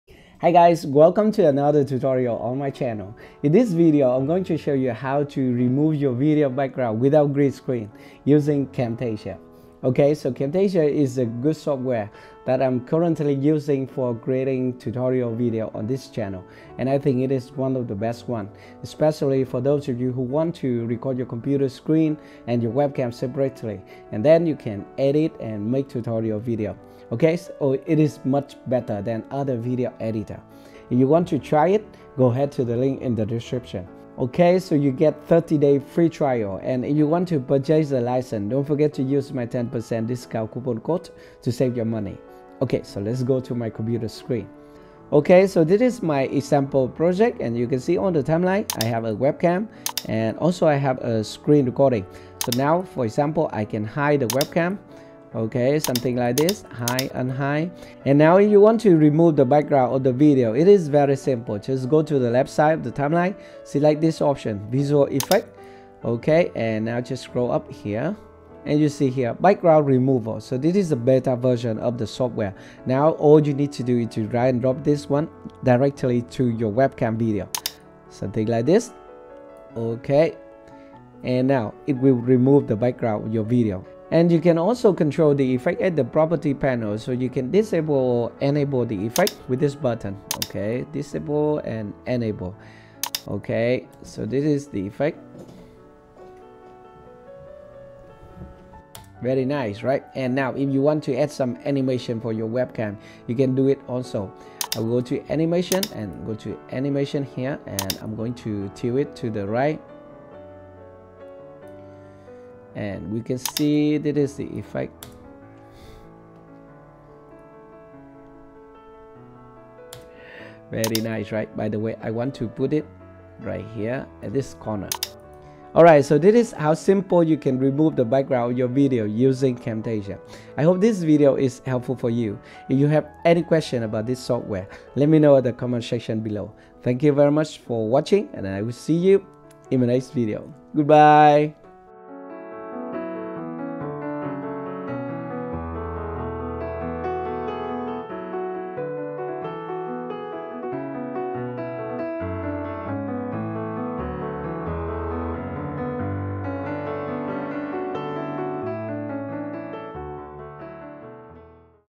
How to Remove Video Background Using AI in Camtasia
This video shows you how to remove video background in Camtasia.